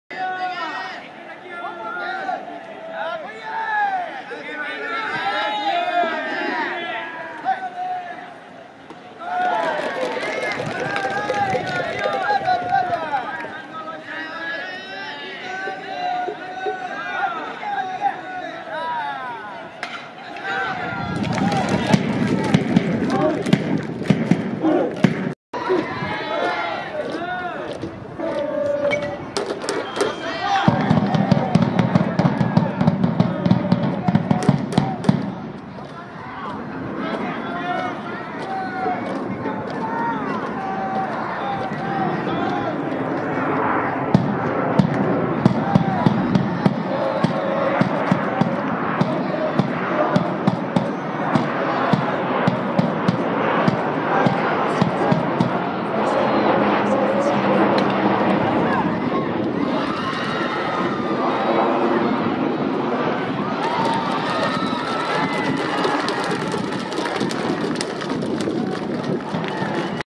川島高校 田岡大樹選手(2017/03/30)
2017/03/30　鳴門オロナミンC球場
徳島県春季大会　川島対徳島商業